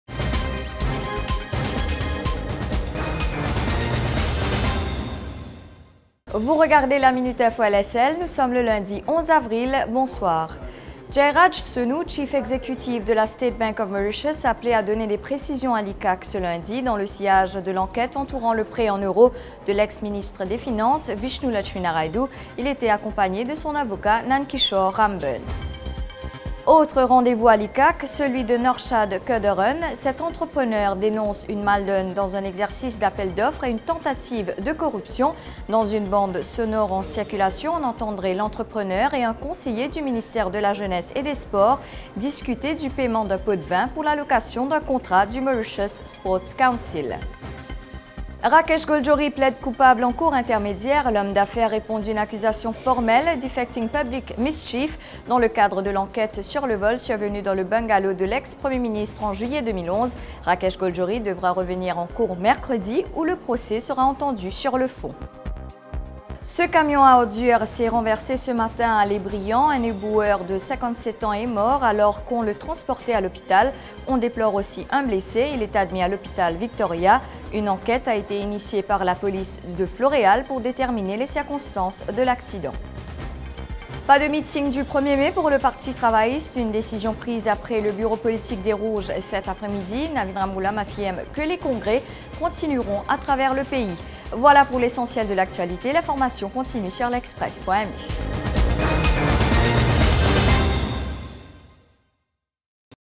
LaMinuteInfo: le Chief Executive Officer de la SBM à l’ICAC
A la Une de LaMinuteInfo ce lundi 11 avril: Jayraj Sonoo, le CEO de la SBM, appelé à fournir des explications au sujet du prêt en euro de Vishnu Lutchmeenaraidoo. Aussi au sommaire: un accident à Allée-Brillant fait un mort et deux blessés.